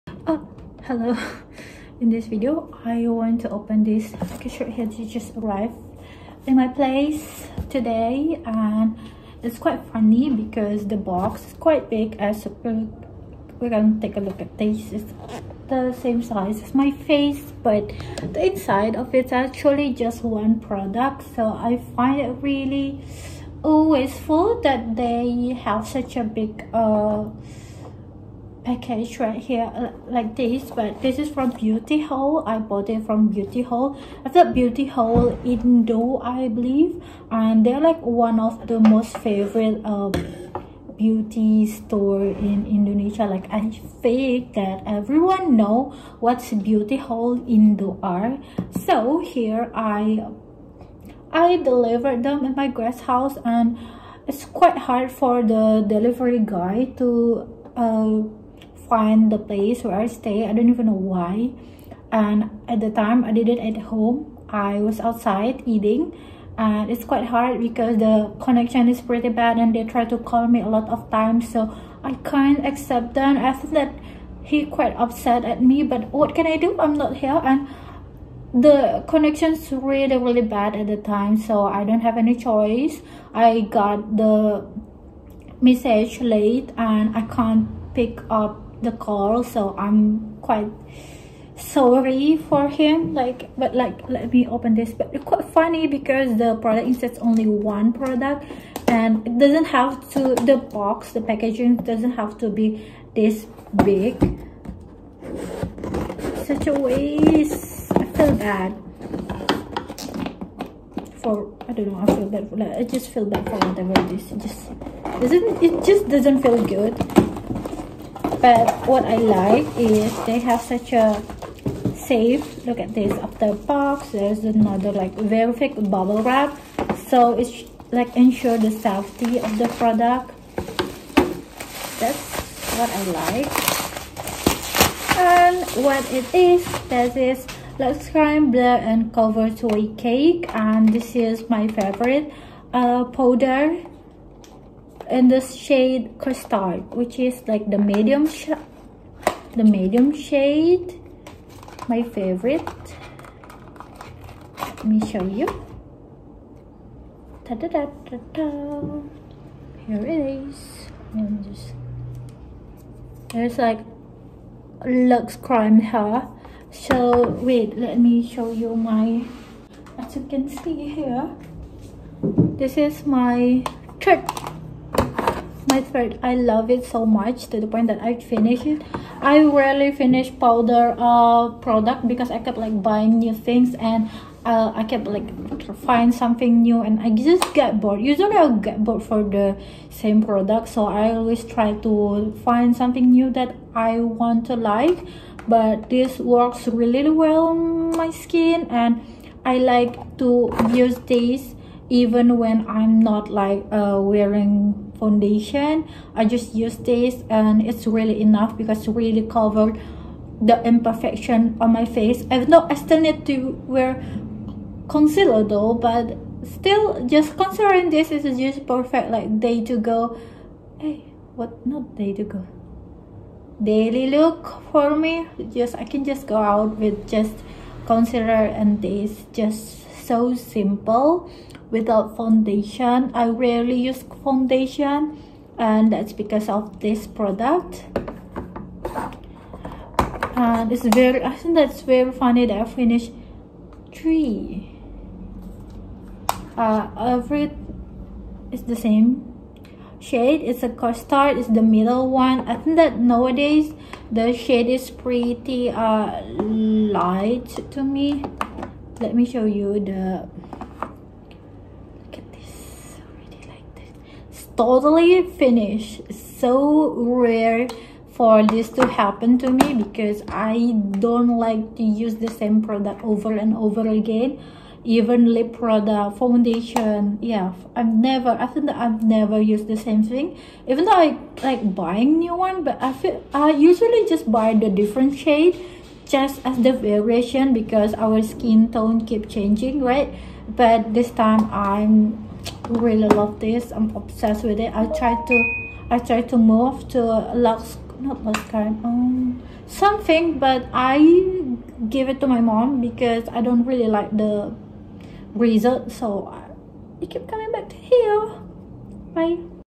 The Third Purchase Of Luxcrime Blur & Cover Two Way Cake Of Mine, I Am Addicted!
Who would have thought that i will love this product this much? Not me 😜
Without me realizing, this is already my third purchase of Blur and Cover Two Wah Cake from our local brand, Luxcrime.